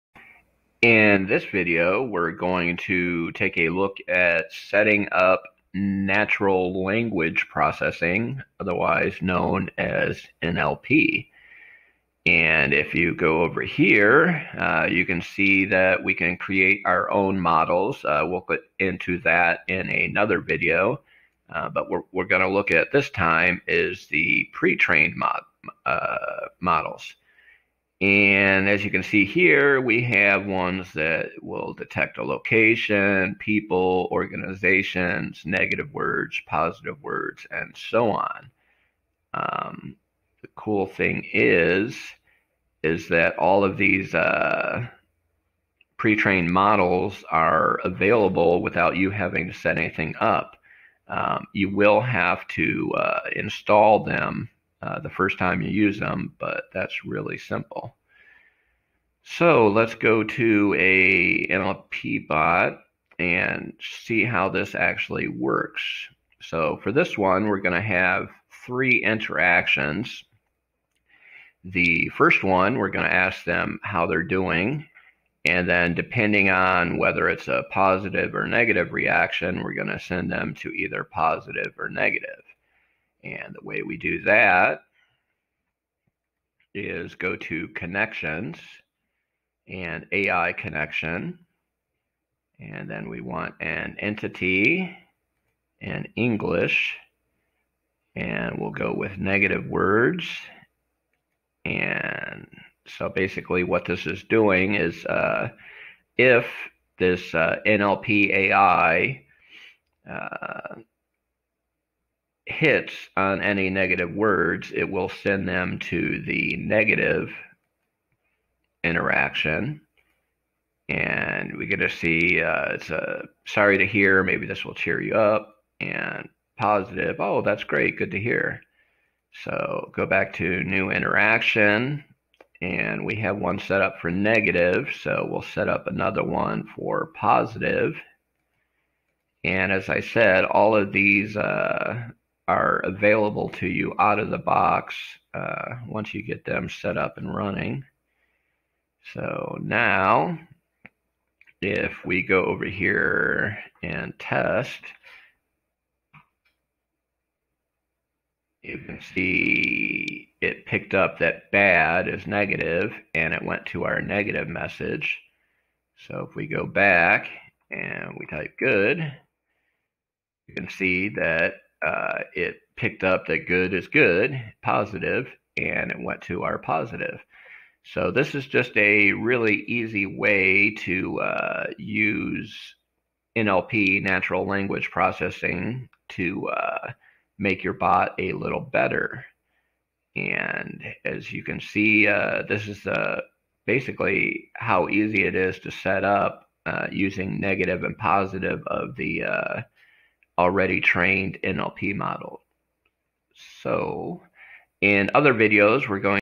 Using pretrained NLP models with SnatchBot
SnatchBot has dozens of pretrained NLP models to help you instantly give your chatbots a sophisticated voice.